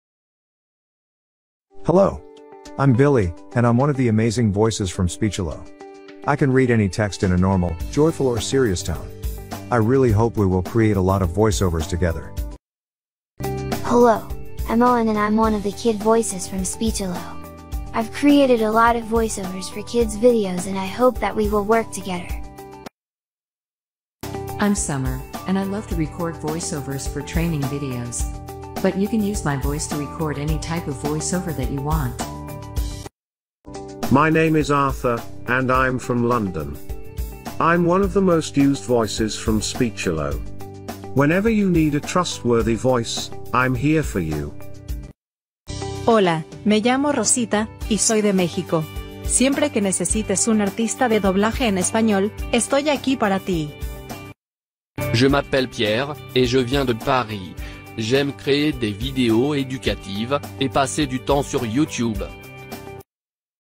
Speechelo Demo Voices - Listen To Speechelo Voices
Speechelo Voice Demos Speechelo is the latest text to speech web-based tool for creating high quality voice-overs.